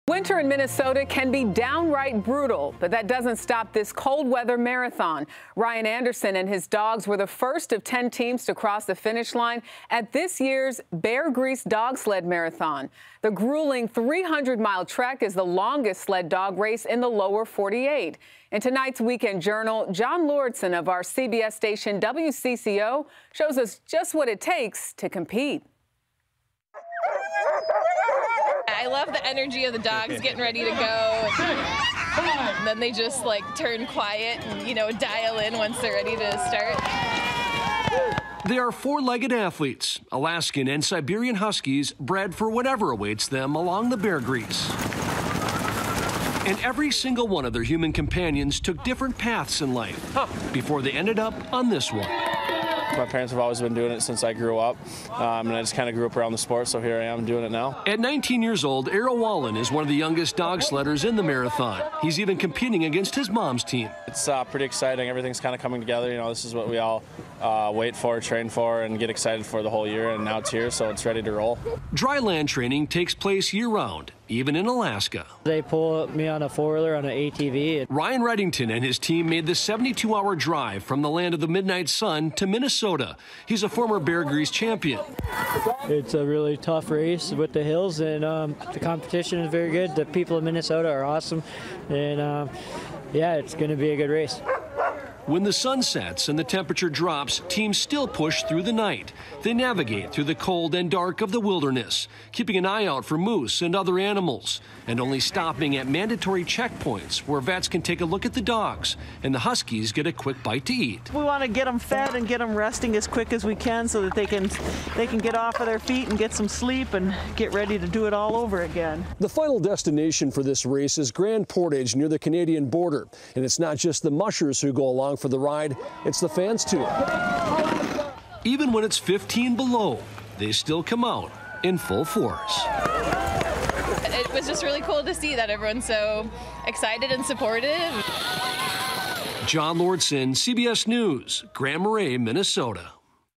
A look at what it takes to compete in the 300-mile Beargrease Sled Dog Marathon
Every year, dog owners from all across the nation gather in Minnesota to compete in the John Beargrease National Sled Dog Marathon. CBS Minnesota’s John Lauritsen shows us what it takes to compete.